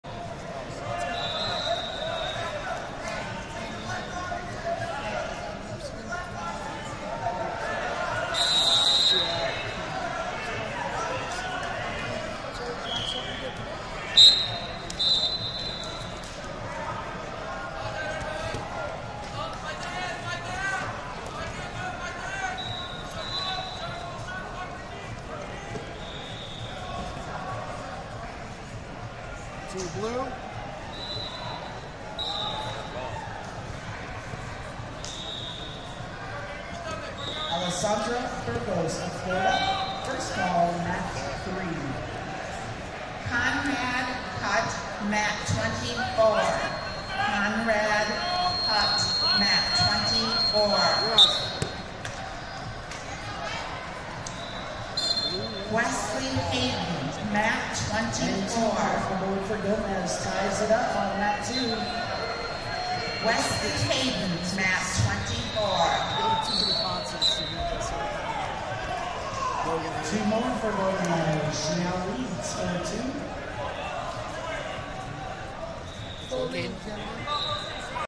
52 Kg Rnd Of 64 - Vaughn Geisendorf, Aniciete Training Club Vs Logan Haney, Howe Wrestling School,
Flosports-partner-access/2023 US Open Wrestling Championships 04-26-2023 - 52 Kg Rnd Of 64 - Vaughn Geisendorf, Aniciete Training Club Vs Logan Haney, Howe Wrestling School, LLC D2ae